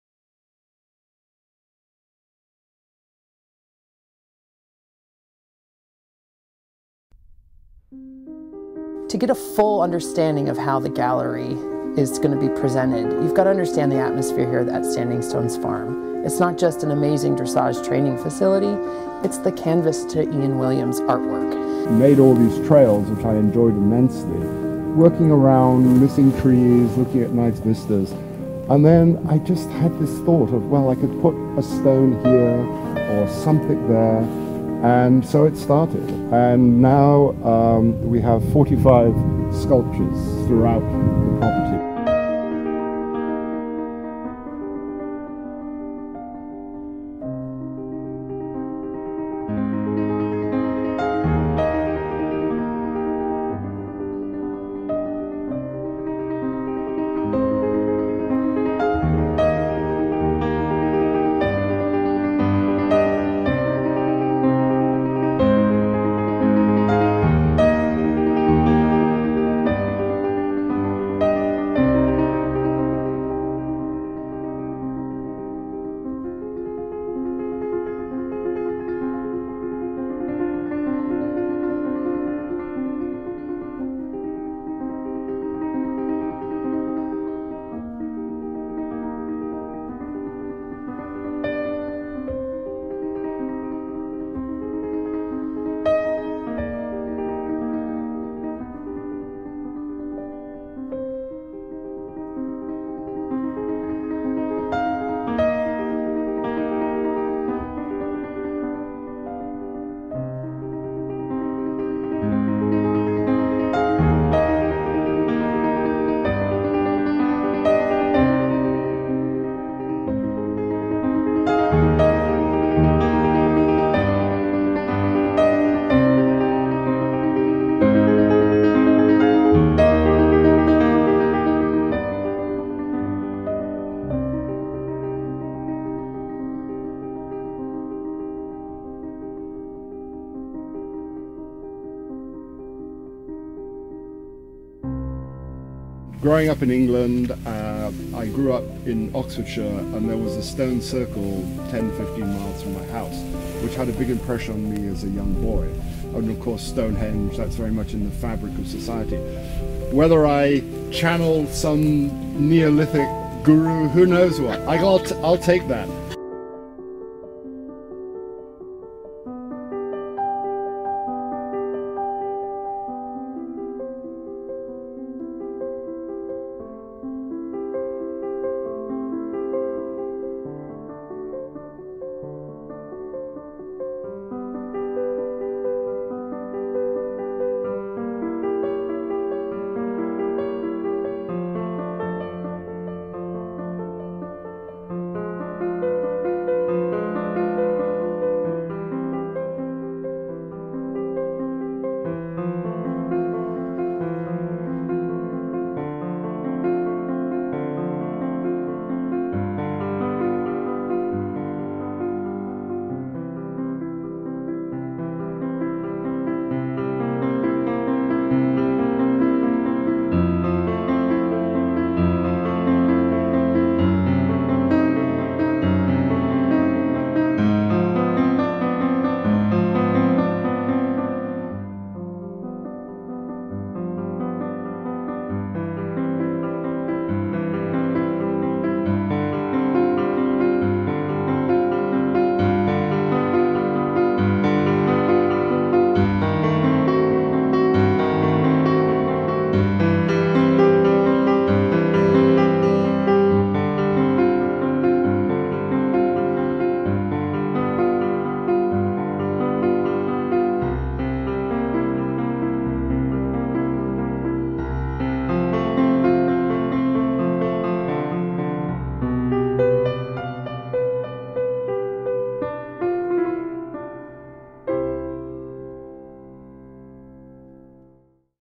The Gallery by Ian Williams mov
A brief video made by Skye Reno and Jordan LaPlaca describing the sculptures at Standing Stones Farm